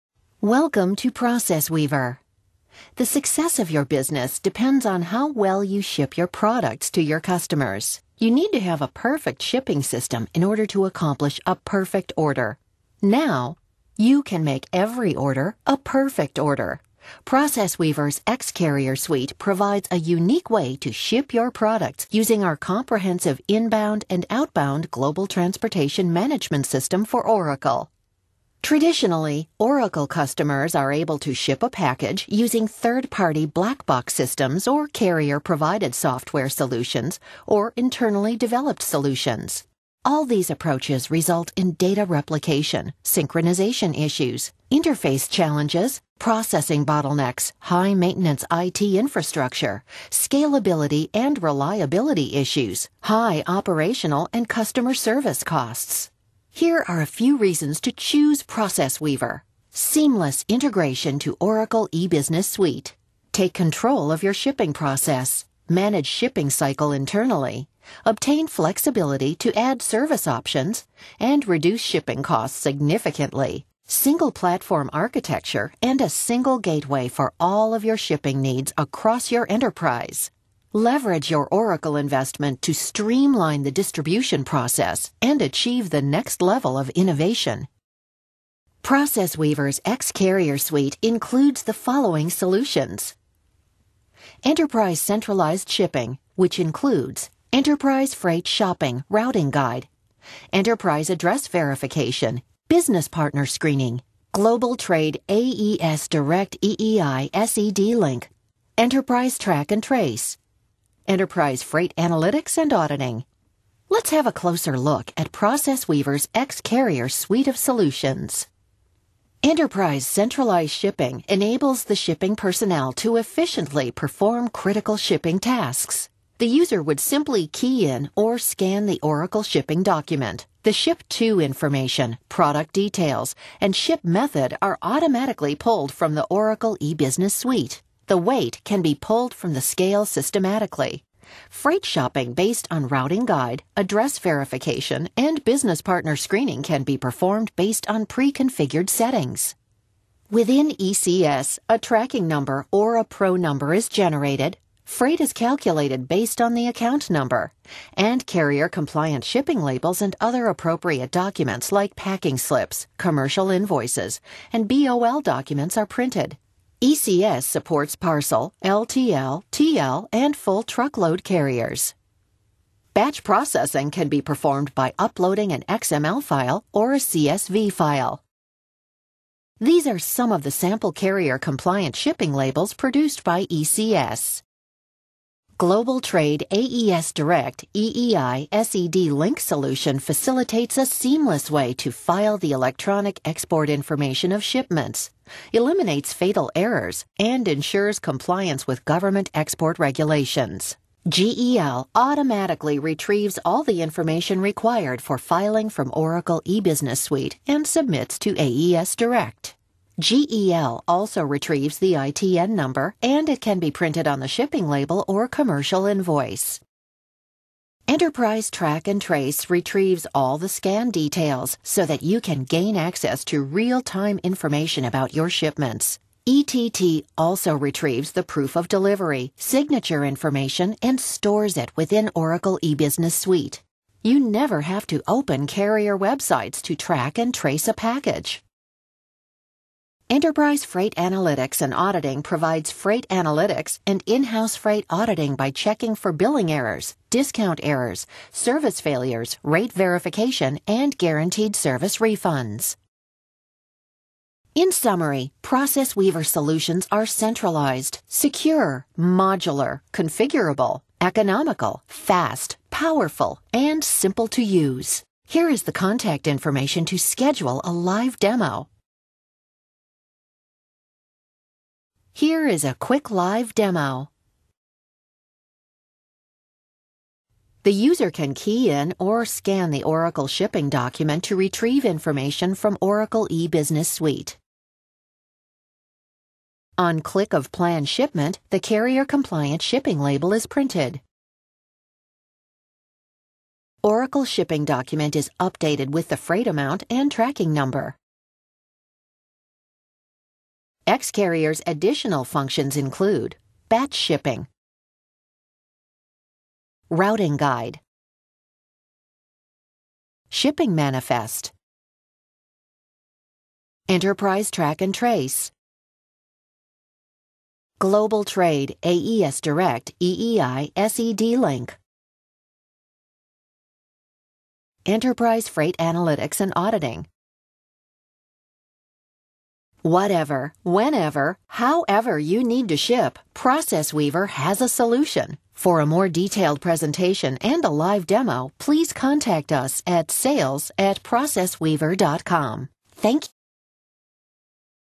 Oracle Shipping Software - ProcessWeaver
International shipping involves a lot of paperwork, but with oracle shipping  Software  shipping becomes a whole lot easier. It is critical that we ensure our packages and items arrive at desired destinations successfully by making sure paper work and other requirements are done correctly and on time. ProcessWeaver's oracle shipping systems provide the most detailed multi-carrier manifesting shipping procedure that enables shipment processing and handling to be executed straight from Oracle E-Business Suite Landscapes